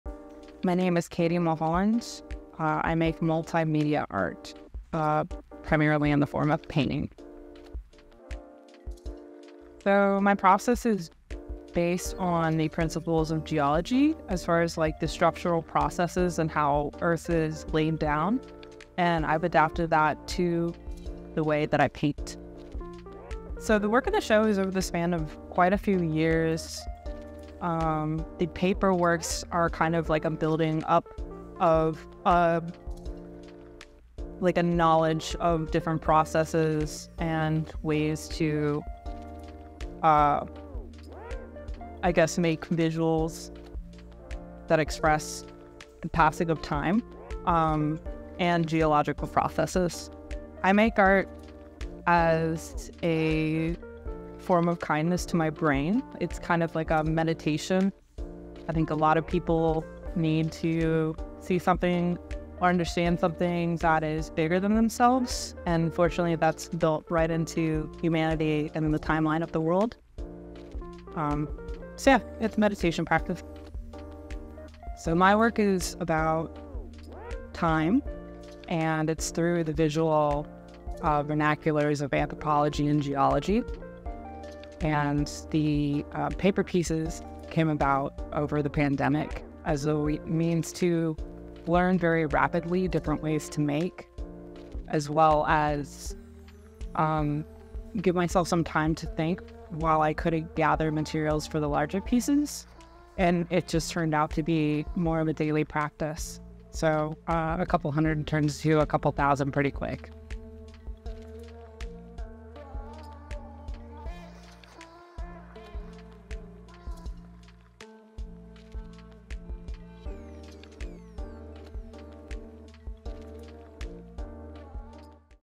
Kate Mulholland Emerging Artist Volume IV
Kate Mulholland is an exciting Houston artist currently on show at Foltz Gallery for our Texas Emerging IV.
Watch the @kajmulholland interview to learn more about the artist and her craft.